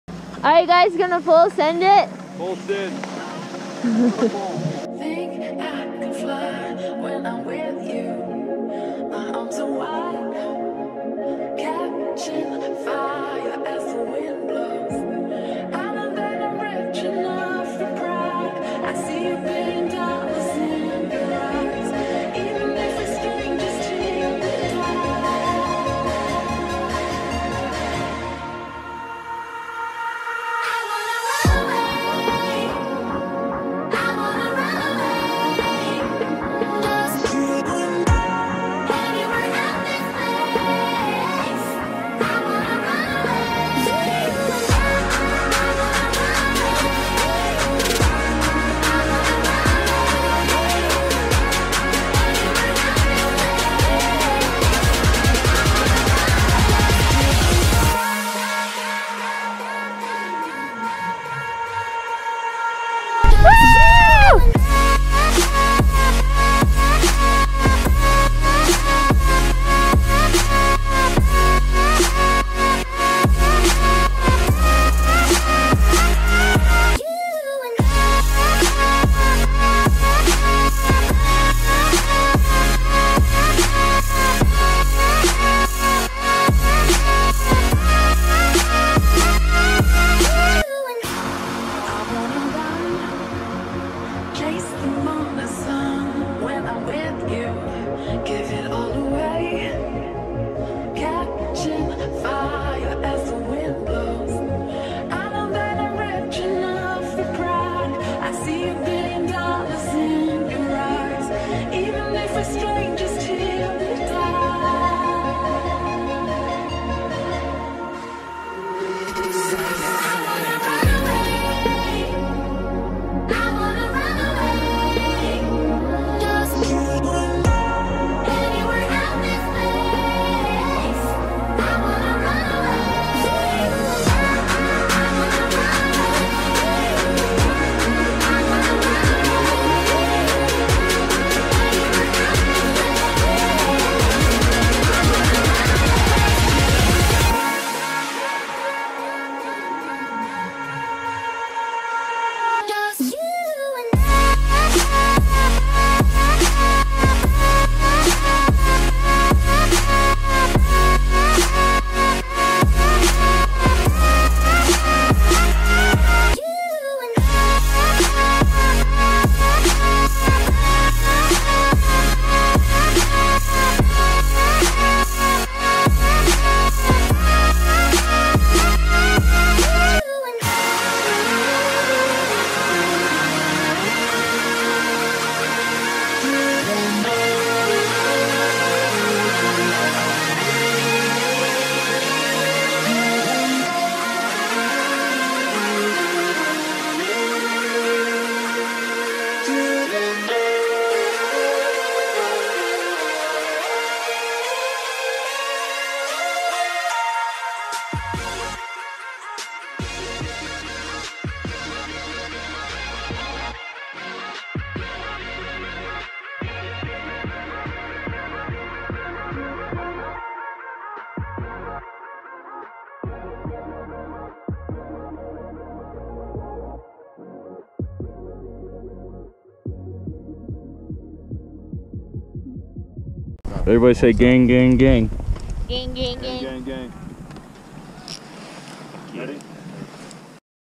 Vail, Colorado | Epic Skiing & Snowboarding Vacation
Our New Year's vacation in Vail, Colorado! Had a blast shredding the slopes!